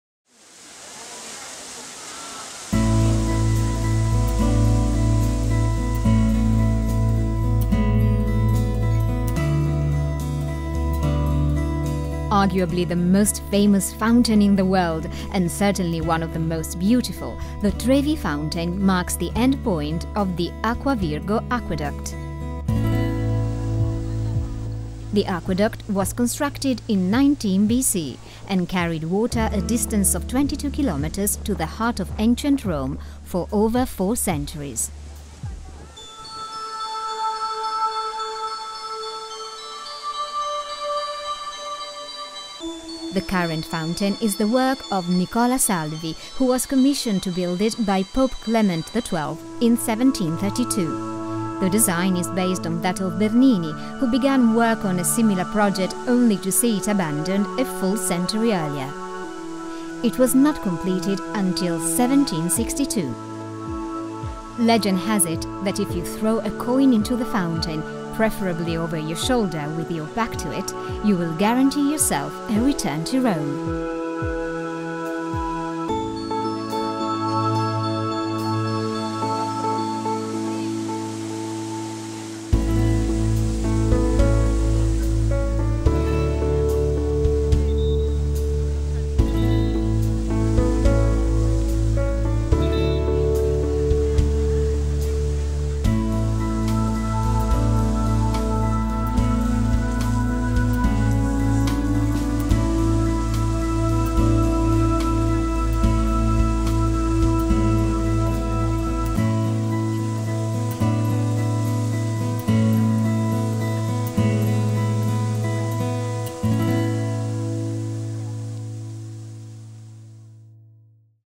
Trevi Fountain, Rome, Italy - 1080HD
The Trevi Fountain is one of Rome's most iconic attractions and it's easy to see why so many romances began here or were renewed around this amazing and beautiful structure. The fountain was completed in 1762 but the water that flows through it comes from a Roman aqueduct that dates back to the 1st century BC. Tradition has it that by tossing 3 coins into the fountain you will 1) be assured of returning to Rome, 2) fall in love with an Italian sweetheart, and 3) marry them. Around €3,000 are collected each day and donated to charity. That's a lot of future weddings!